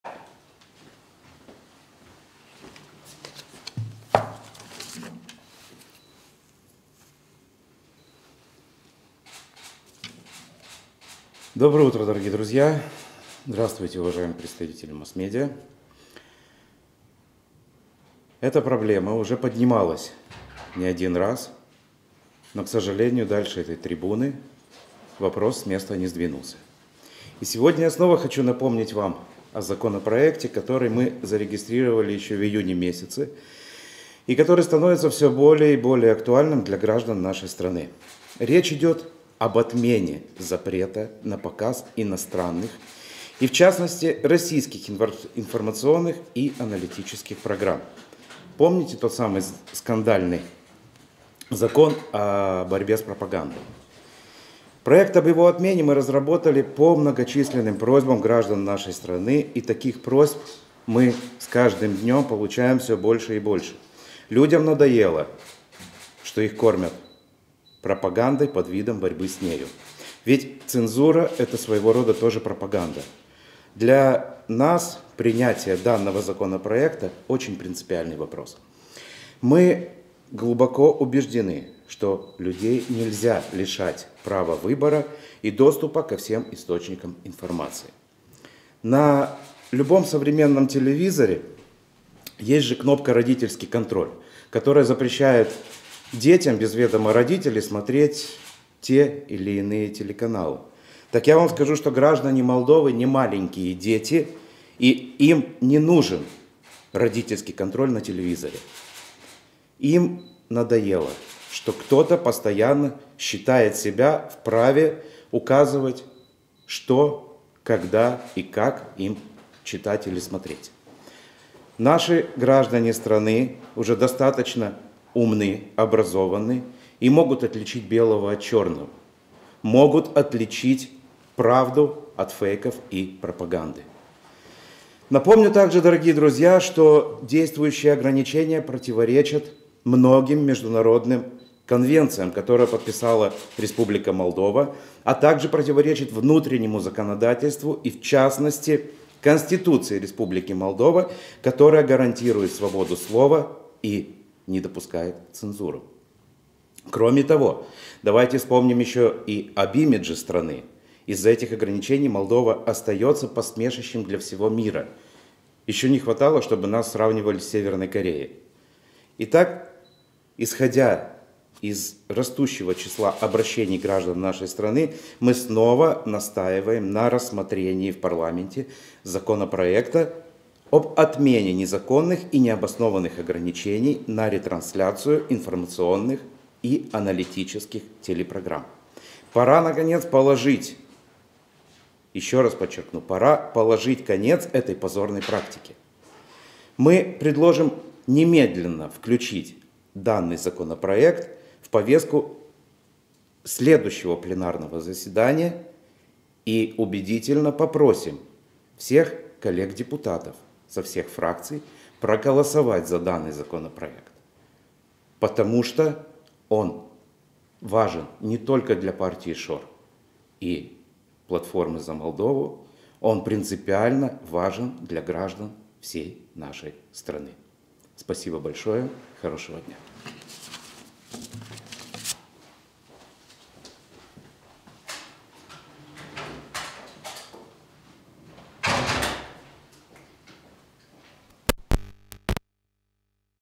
Briefing susțiinut de deputatul fracțiunii „Partidul ȘOR”, Igor Himici - 2 decembrie 2020
Briefing de presă susțiinut de deputatul fracțiunii parlamentare „Partidul ȘOR”, Igor Himici - 2 decembrie 2020